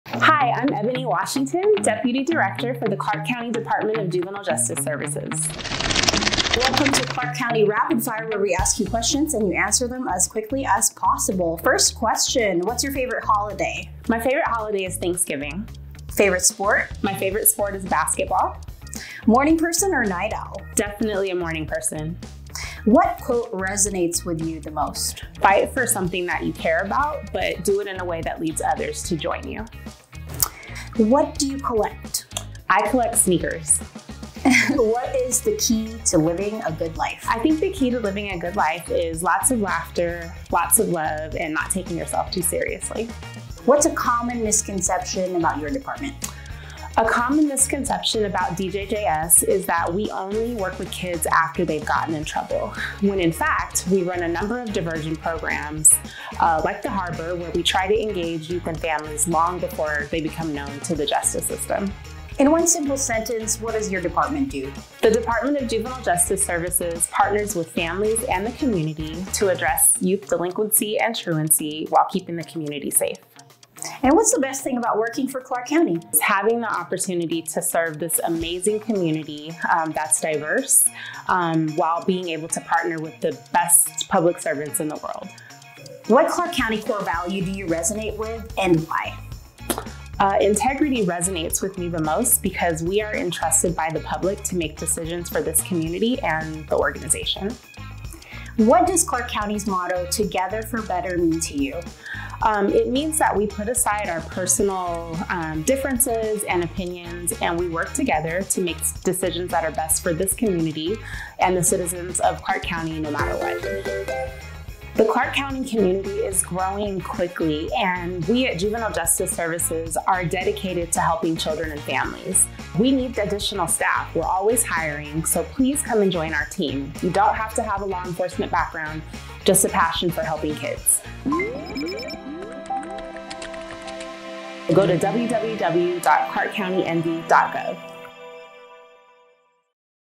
Clark County Rapid Fire: Eboni Washington
Meet Eboni Washington, an early riser & sneakerhead! 🌅👟 She's not only passionate about her kicks, but also about her role as one of the Deputy Directors at Clark County's JuvenileJustice Services. Hit play ▶️ to learn about Eboni & the work her team does.